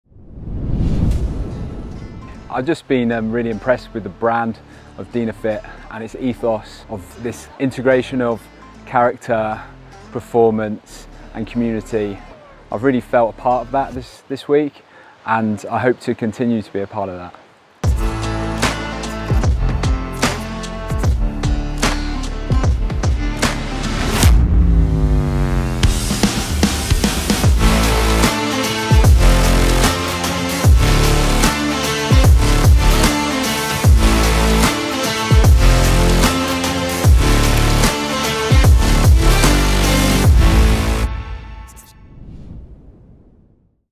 Ultra Camp | YOUR next challenge | DYNAFIT
Trail Running is your passion? Speed is in your DNA and you would like to improve your running skills? Apply now for our Ultra Camp and get ready for your next challenge.

Join extensive trail sessions with like-minded runners, learn all about training theory and test our latest ultra collection.